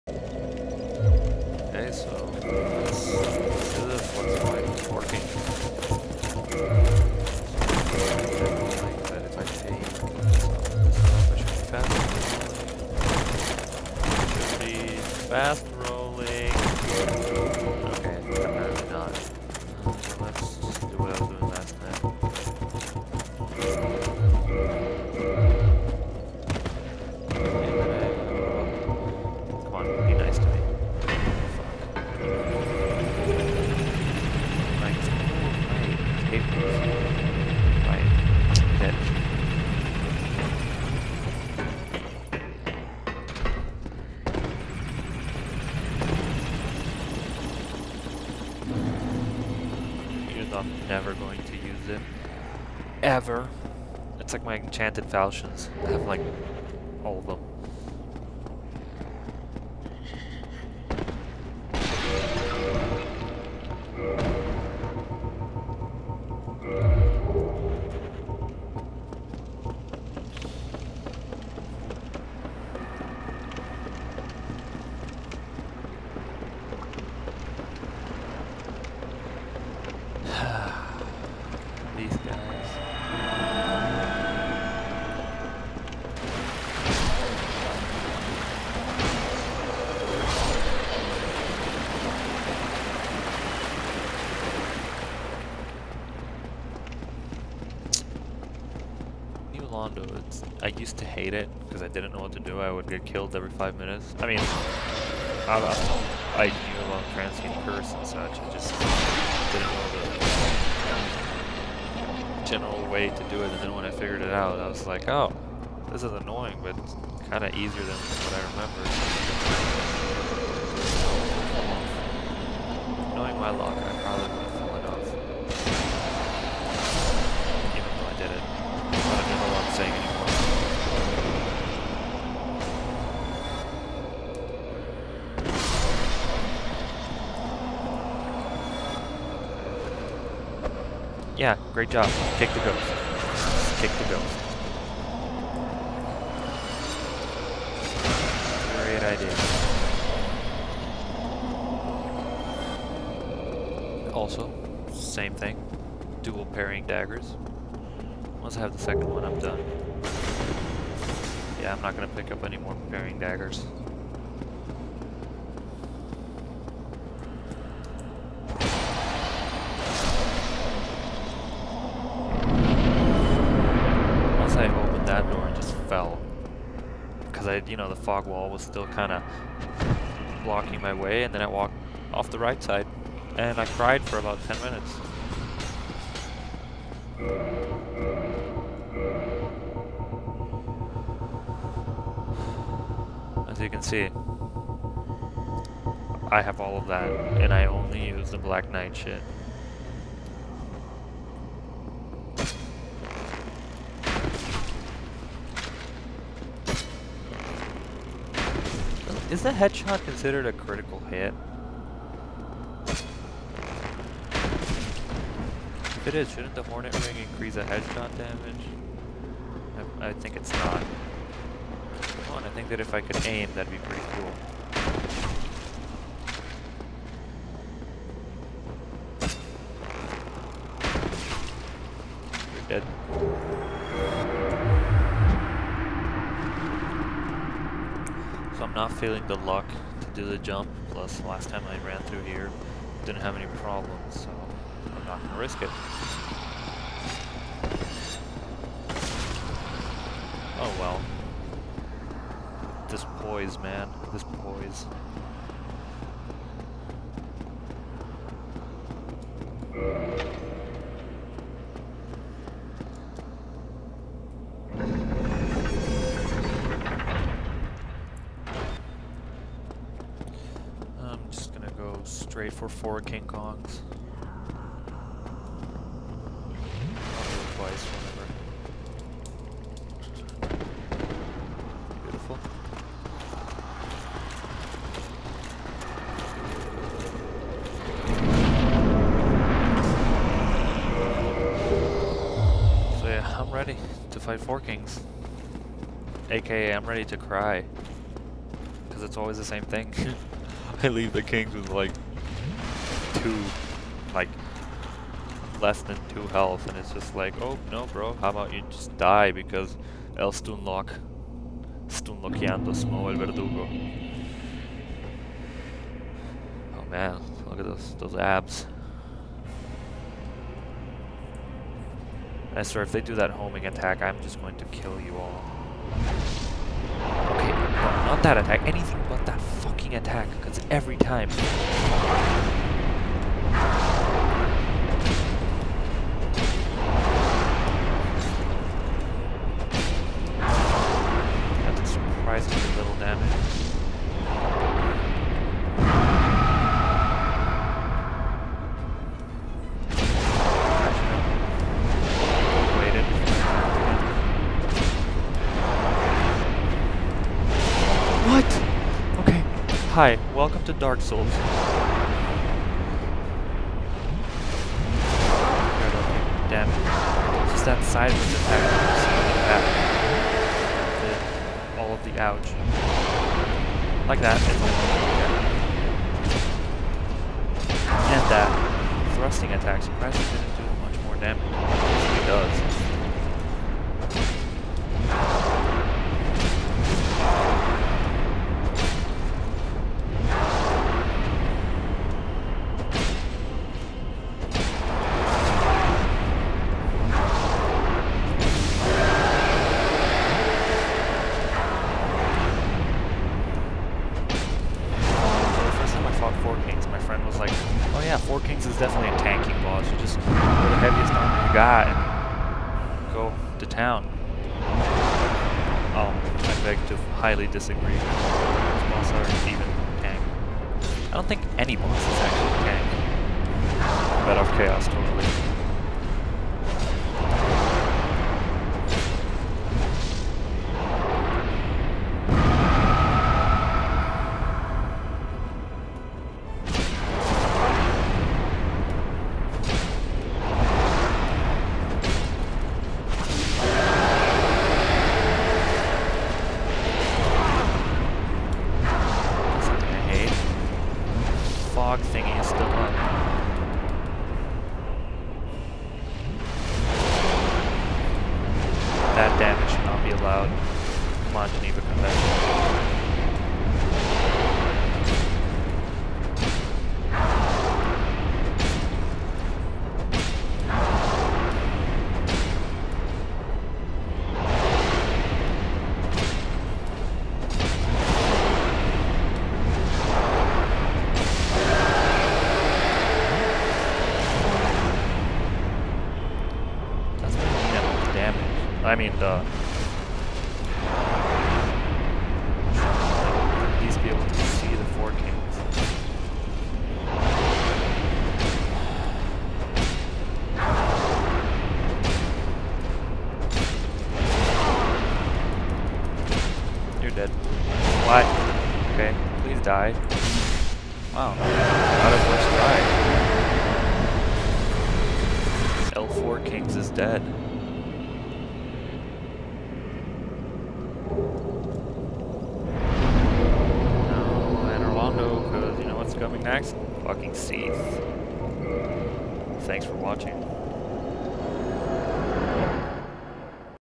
For Kongs NG+5
I just decided to beat the game as many times as i can, and then decided to beat Four King Kongs and record it. lel